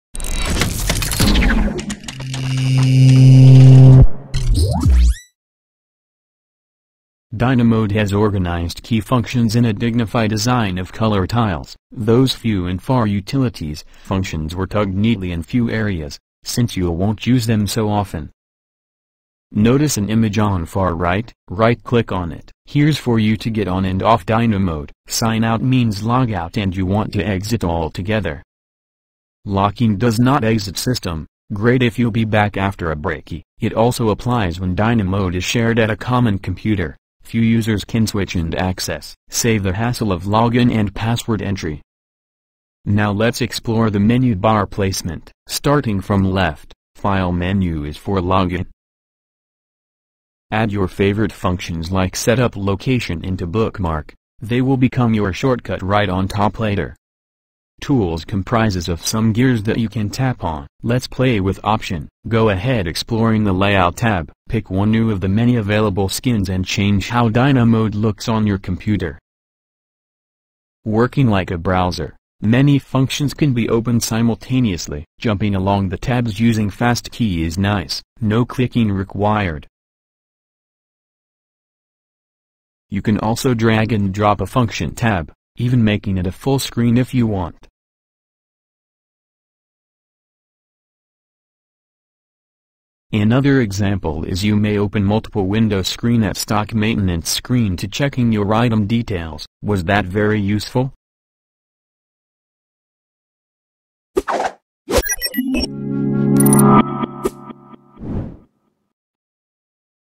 1.3 Navigation Menu Bar
Learn about the essential of home screen menu bar & window behaviour is great way to start using DynaMod system.
- Sign in & out of system or have it lock
- exploring some functions & tools in menu bar
- learn to move amongst multi window tabs, & change the display like cascade & others